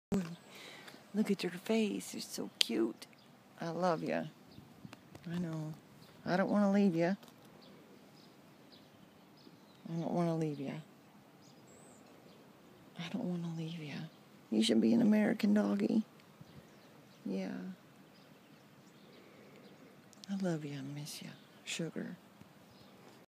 Galley Dumpster Dog Needs Transport to USA
Please consider donating to help these three Sicilian dogs get to their forever homes in the States!